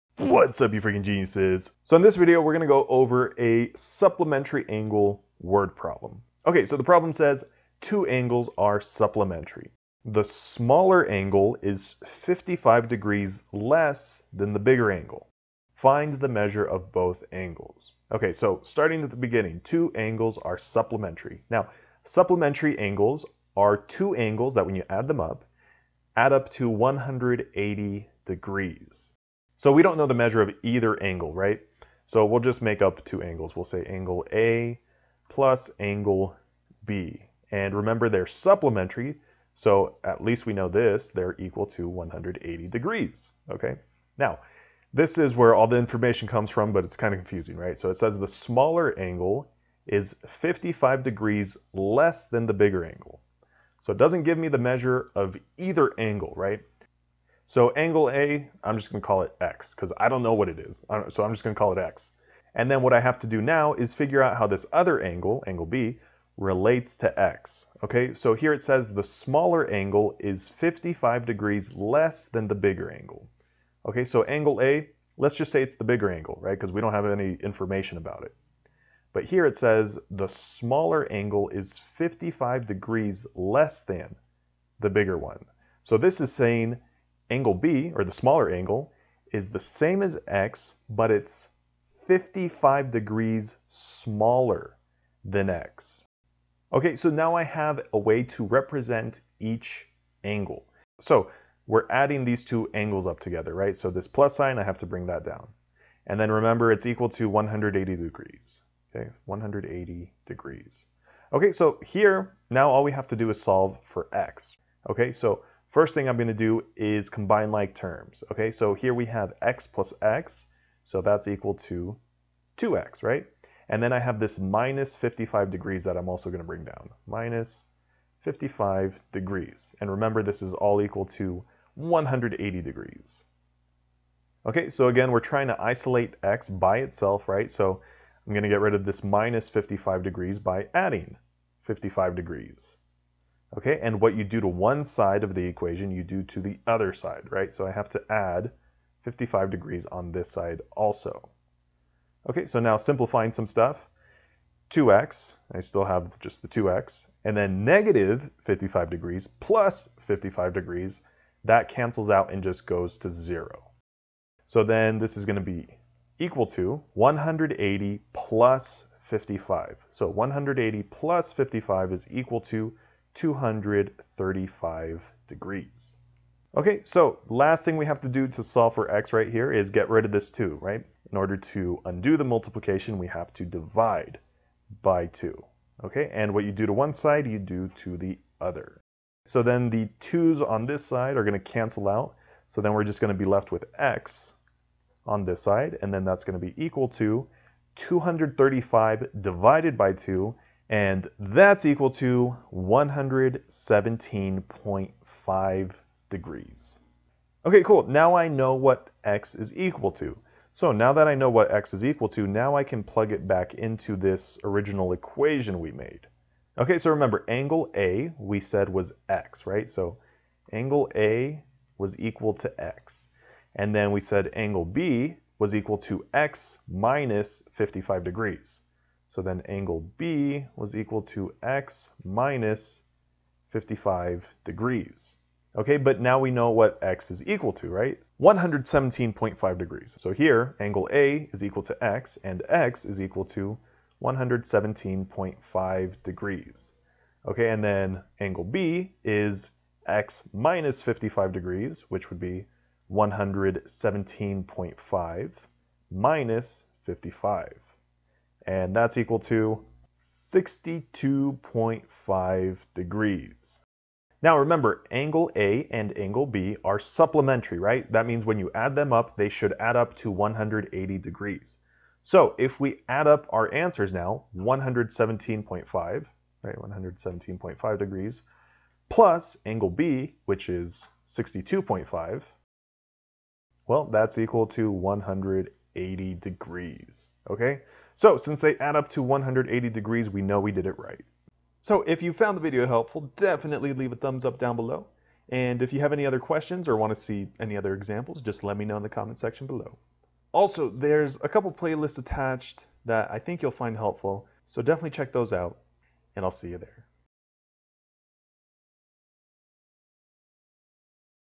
Supplementary Angle Word Problem | Geometry | Eat Pi
In this video, I teach you how to set up and solve a supplementary angle world problem.

Problem: The smaller angle is 55 degrees less than the larger angle. Find the measure of both angles.